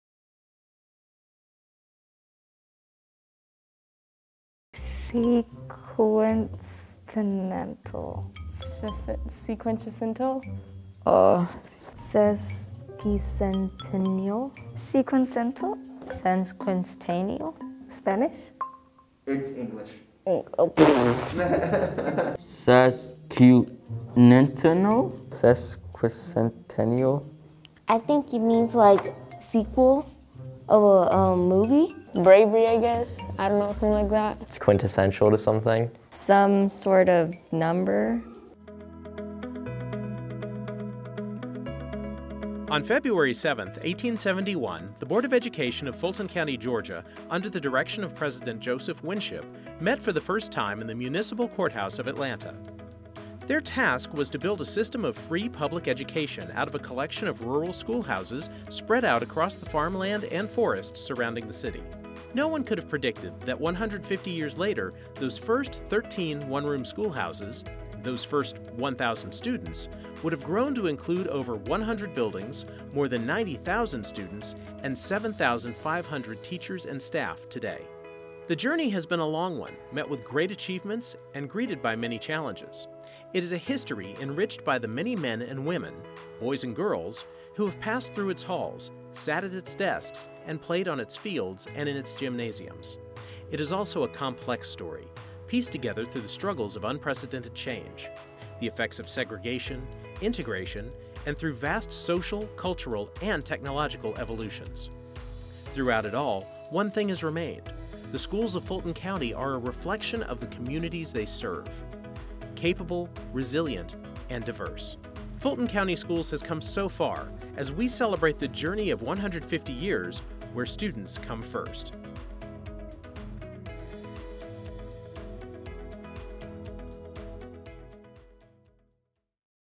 Fulton County Schools - Sesquicentennial Celebration - Intro Video
In February of 1871, the Fulton County School System was established in a courthouse in downtown Atlanta. Over the past one hundred fifty years, it has produced a vibrant history of unprecedented change and unrivaled growth.  We are celebrating the historic journey by looking through the lens of this enduring legacy.